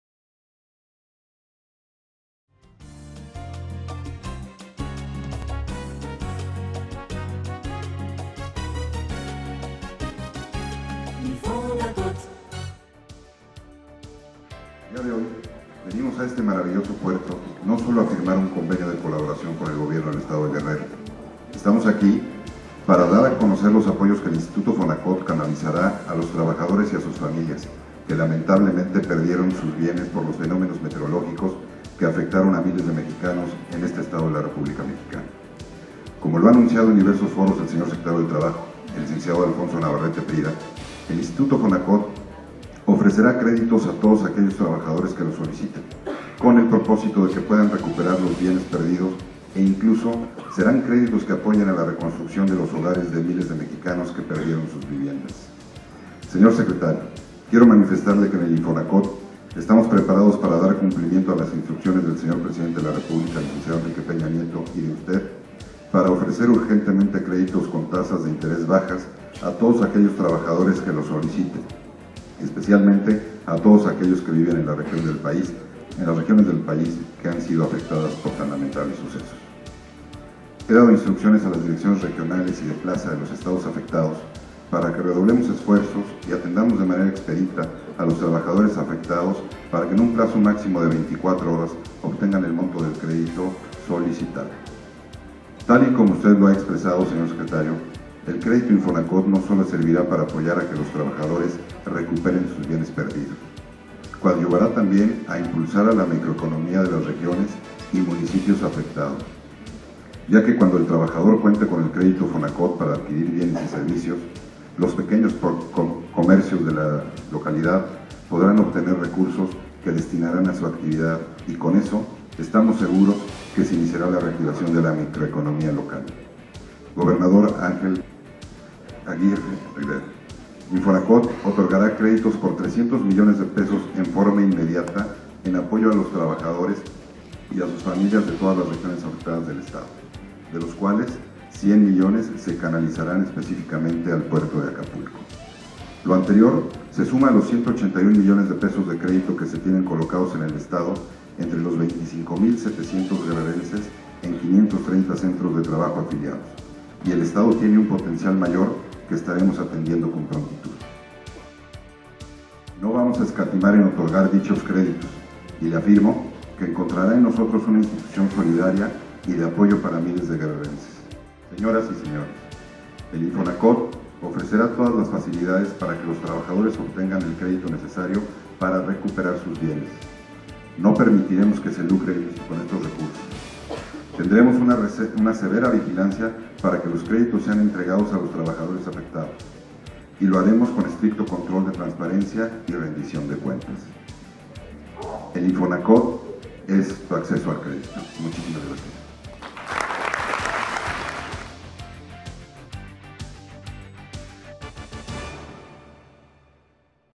Firma de convenio con el Gobierno de Guerrero
El Director General del INFONACOT César Martínez Baranda firmó convenio con el Gobernador del Estado de Guerrero Ángel Aguirre, con el propósito de apoyar a los trabajadores afectados en los recientes tragedias climatológicas en el Municipio de Acapulco.